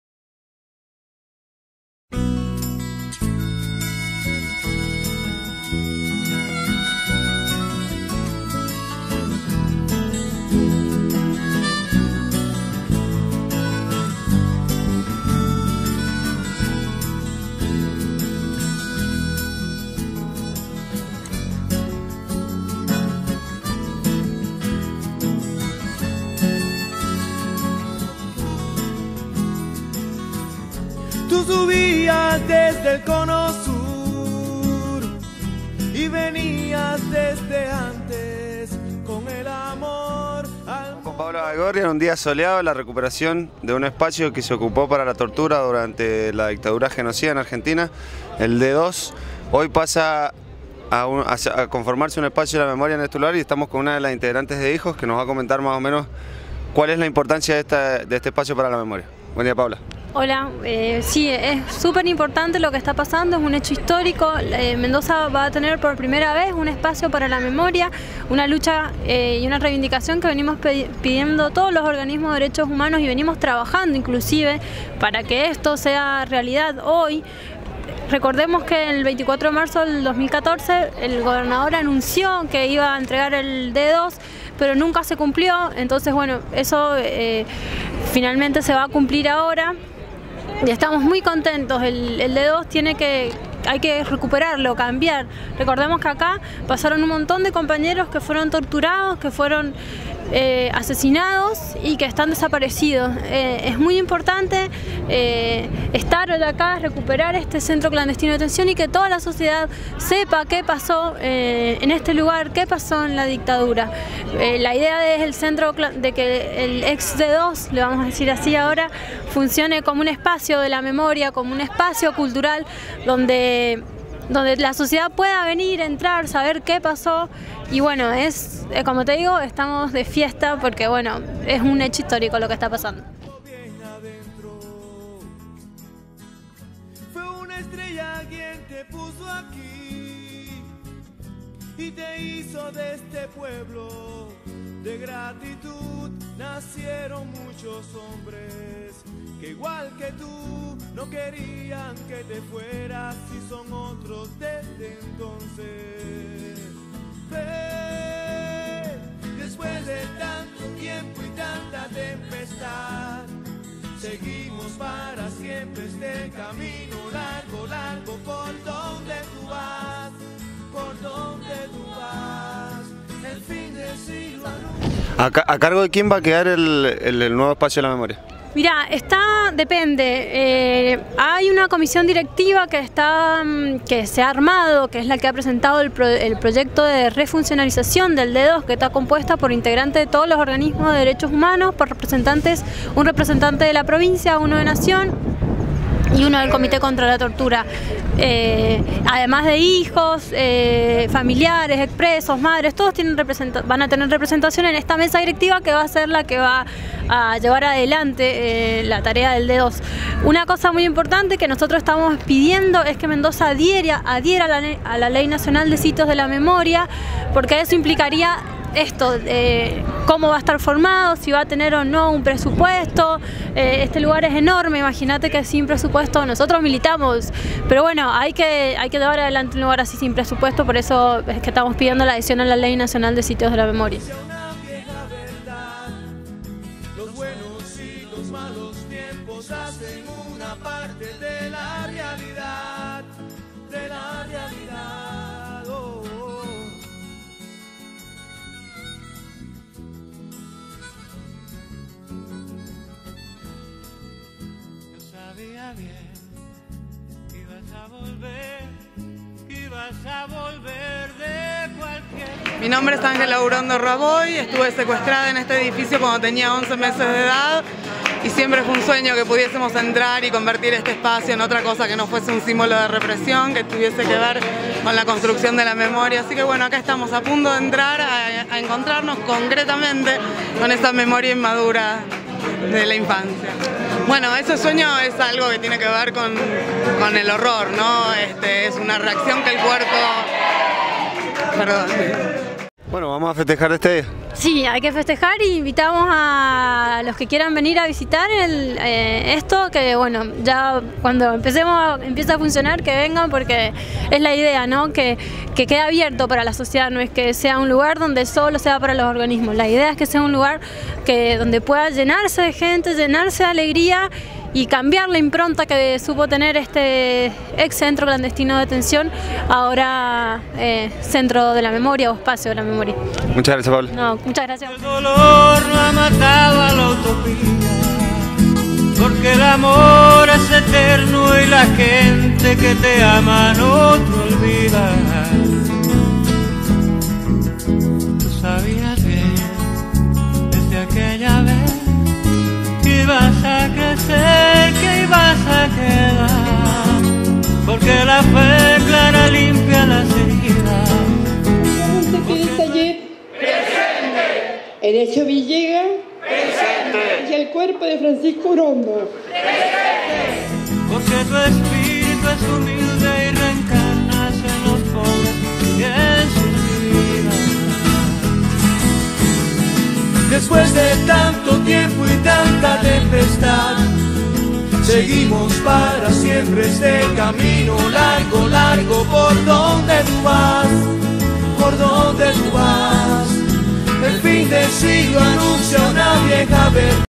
Recuperación del Ex D2 en Mendoza
El sábado 12 de septiembre y después de  esperar por más de una año la promesa del gobernador de la provincia.
El ex-D2 funcionó como centro clandestino durante la última cívico militar de nuestro país.
Así la provincia de Mendoza contará con un espacio para la memoria y para continuar con la lucha de los que faltan.